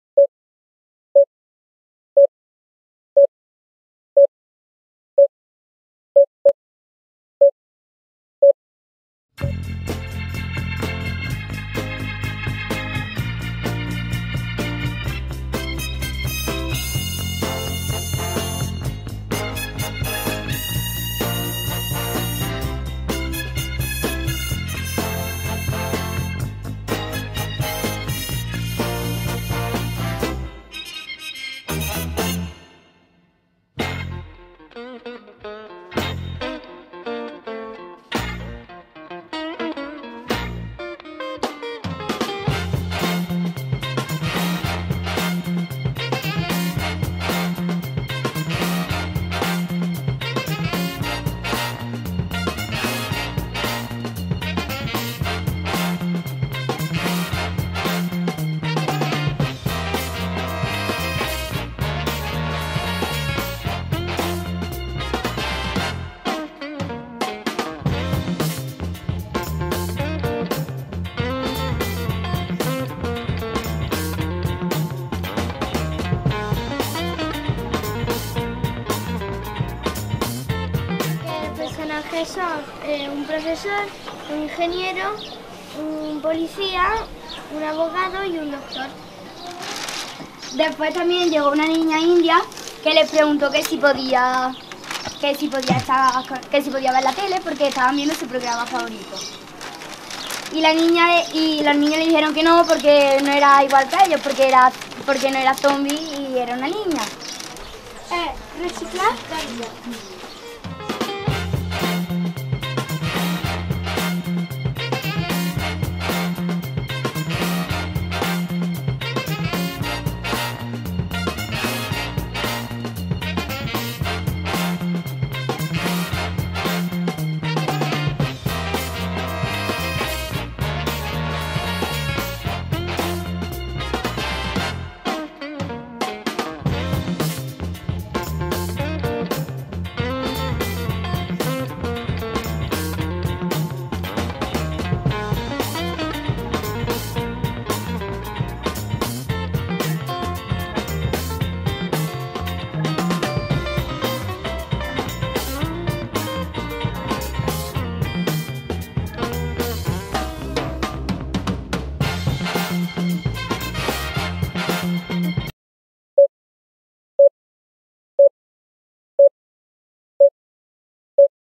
Campamento audiovisual infantil: making of día 2
Este campamento es un espacio para  compartir, participar y construir con otras personas desde el respeto y la riqueza de la diversidad.
En este segundo día comenzamos a pensar nuestra película de animación sobre el Bien Común.